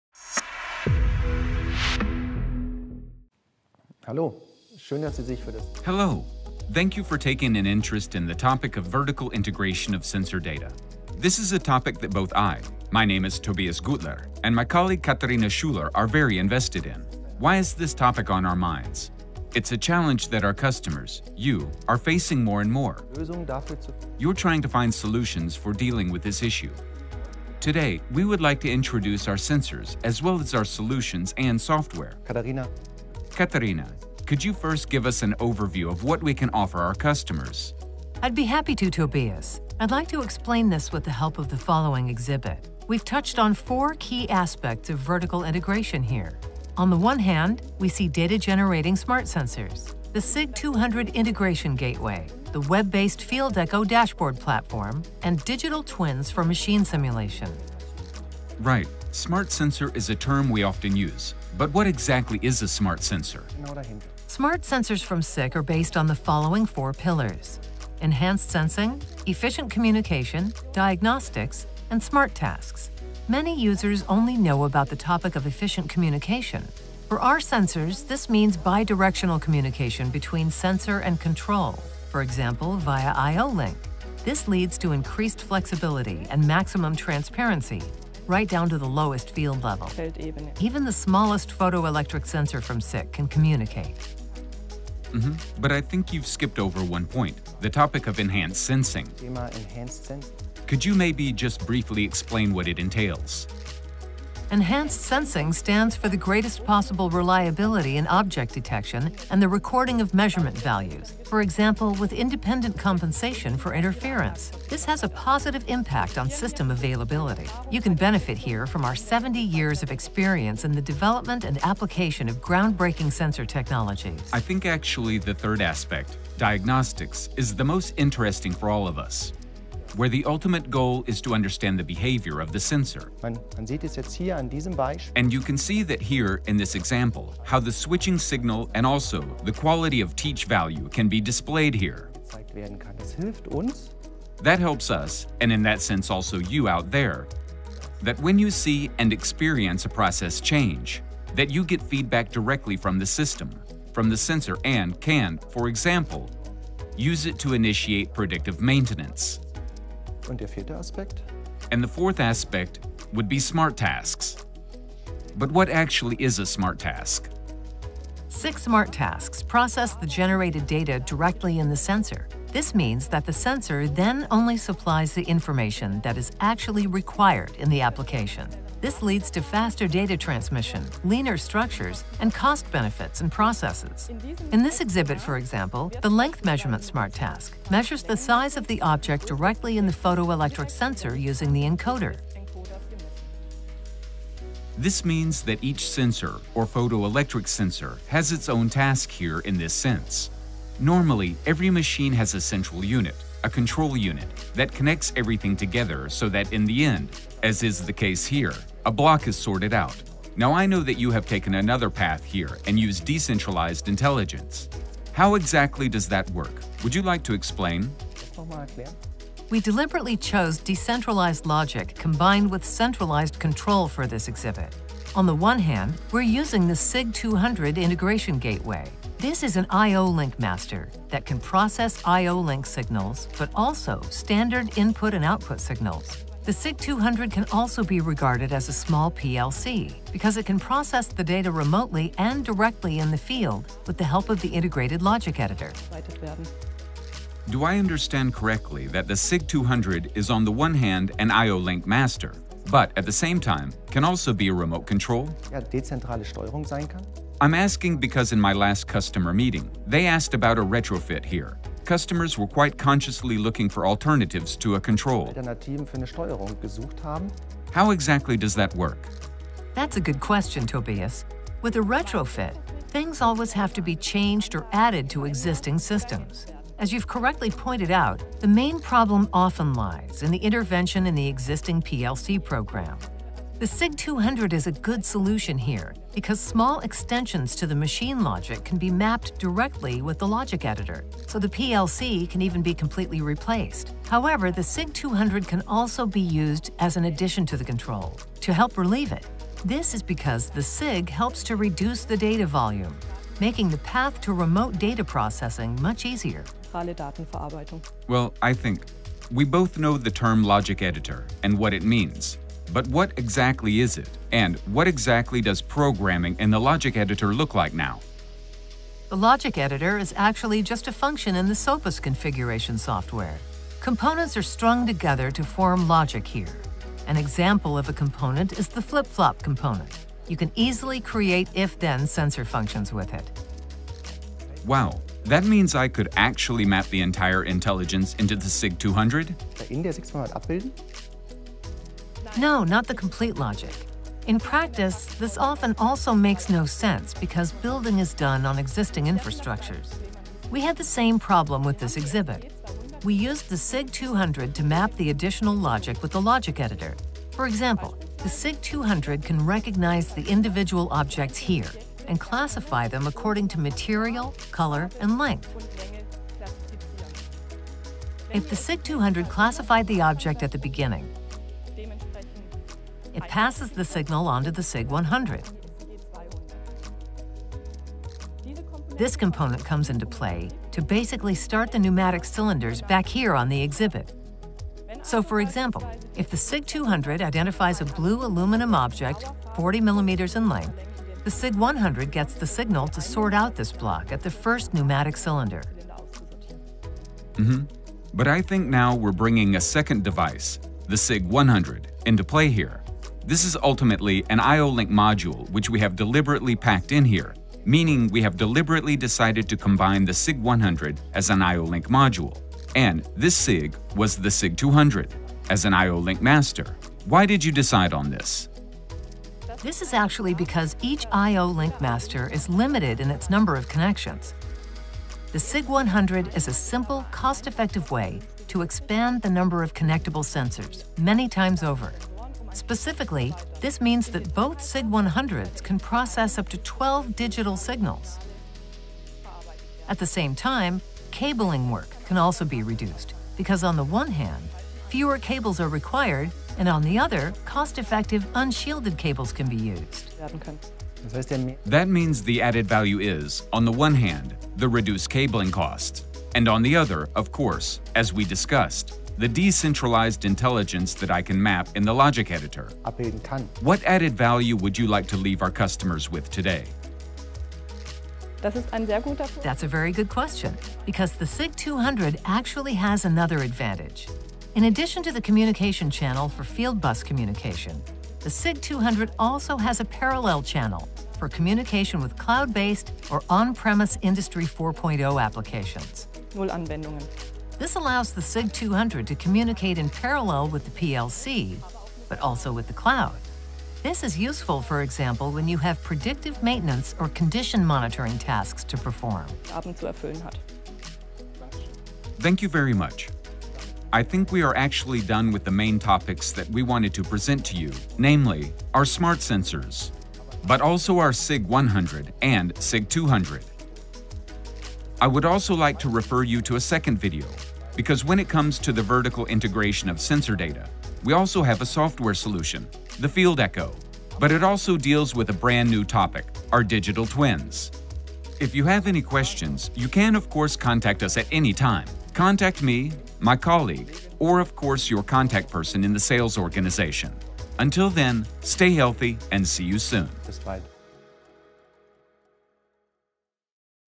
Smart Sensors for factory automation (1/2): From the shop floor to the cloud | SICK AG
Vertical integration of sensor data - how can sensors intelligently generate data? And how can objects of different size, color and material properties be identified in a simple way using Smart Sensors? We will show you this using the SIG200 Sensor Integration Gateway, the web-based dashboard software FieldEcho and the new Digital Twins for machine simulation.